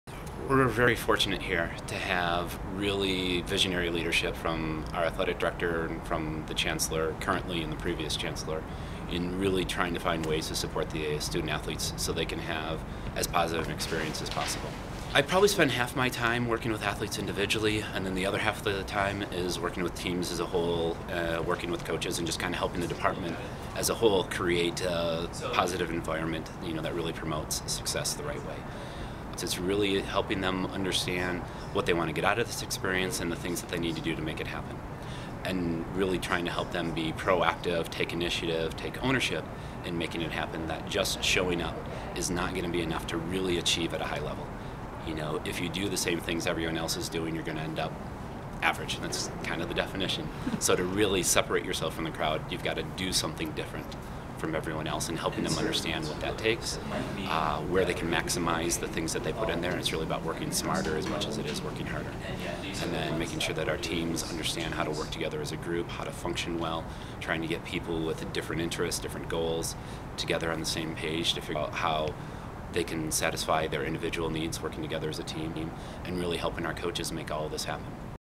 Steve Portenga talks about Sport and Performance Psychology
University of Denver Sport and Performanc Psychologist talks about athlete performance